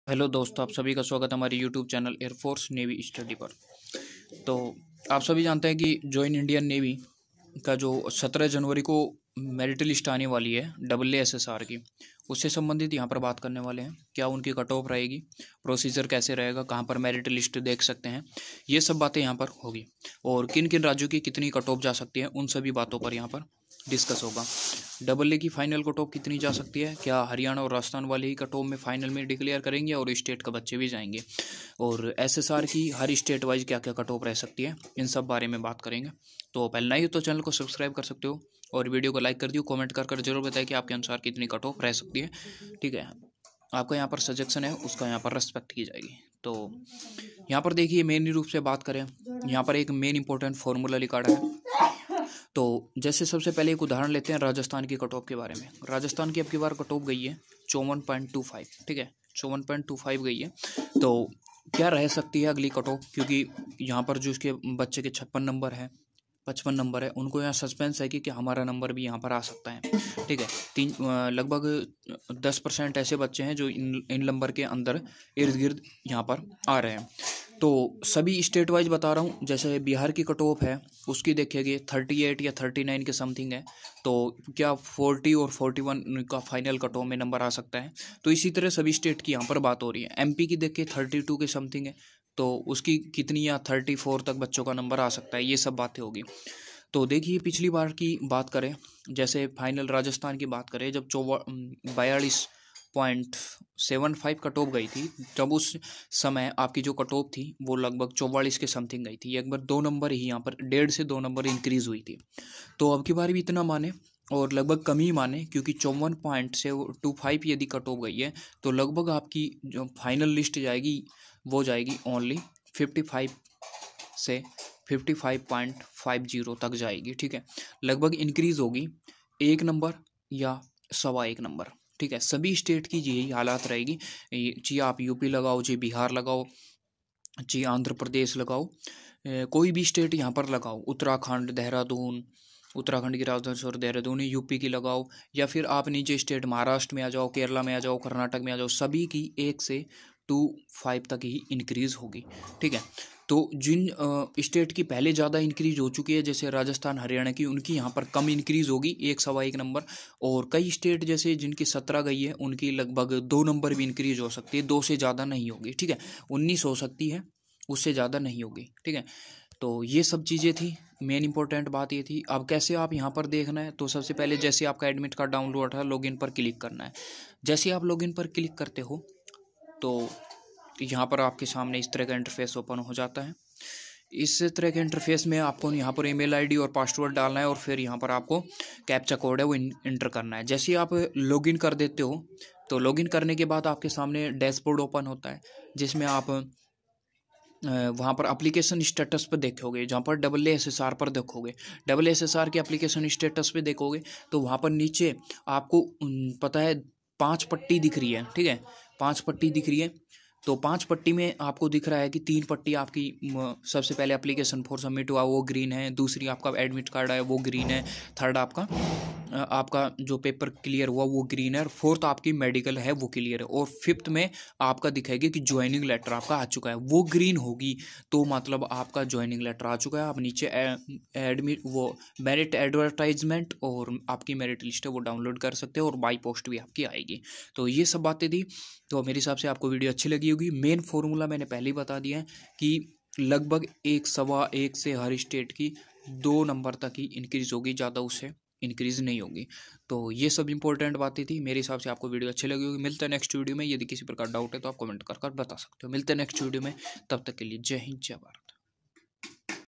NAVY AA, SSR MR FINAL MERIT LIST BATCH-01/2020
hello students

my name is hitesh kumar

welcome on my YouTube channel- airforce navy study

in this video these all topic discuss- es video ma navy aa ssr mr ka final merit list par bat ki gai h. navy aa ssr mr ki final list ki all states ki final cutoff ko discuss kiya giya h. sabda jayada haryana state ka ya other state ka students qualify higa bat ki gai h.